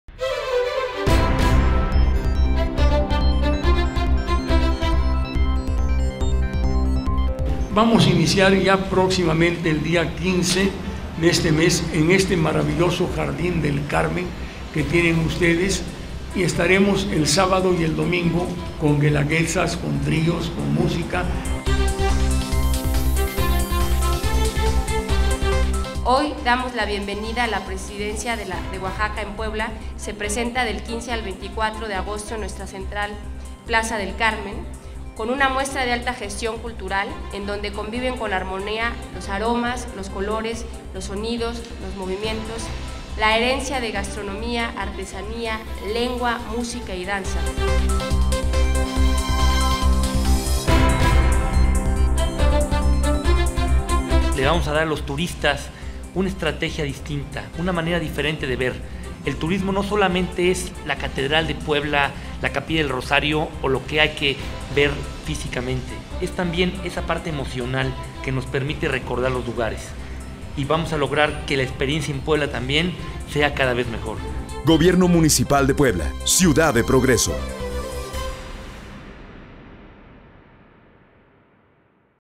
Cultura, arte y tradición Presencia de OaxacaEnPuebla
El Ayuntamiento y el Patronato Oaxaqueño de Cultura Popular presentaron el evento cultural “Presencia de OaxacaEnPuebla” que se efectuará del 15 al 24 de agosto en el Parque del Carmen.

Alejandro Cañedo Priesca, Director de la Oficina de Turismo de Puebla, informó que en esta muestra participarán cerca de 100 artesanos de las ocho regiones del estado de Oaxaca, donde ofertarán sus productos típicos.

Entre las actividades culturales destaca el espectáculo de la Guelaguetza, la participación de los grupos: Trío Monte Albán, D´SANTOS y Trío Fantasía, además de la cantante Ruth Mireles que estará acompañada por la marimba.